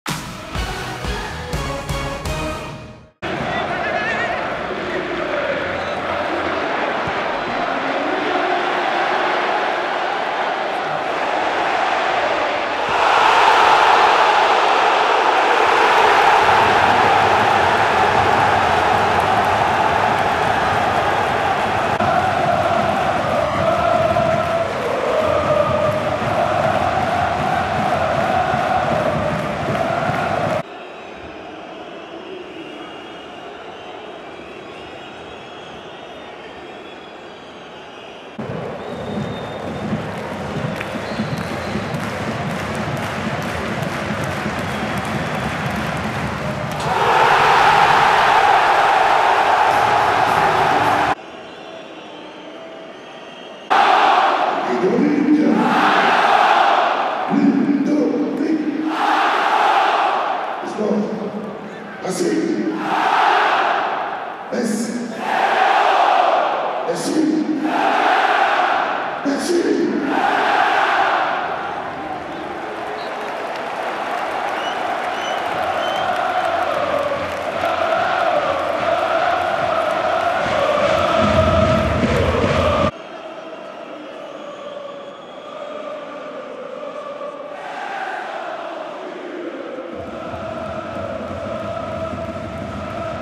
Goal Ludovic AJORQUE (6' pen - RCSA) RC STRASBOURG ALSACE - FC METZ (3-0) 21/22
"RC STRASBOURG ALSACE - FC METZ (3-0) Ludovic AJORQUE (6' pen)  goal. RC STRASBOURG ALSACE vs FC METZ Goals in video

Ligue 1 Uber Eats - Season 2021/2022 - Week 6 STADE DE LA MEINAU - Friday 17 September 2021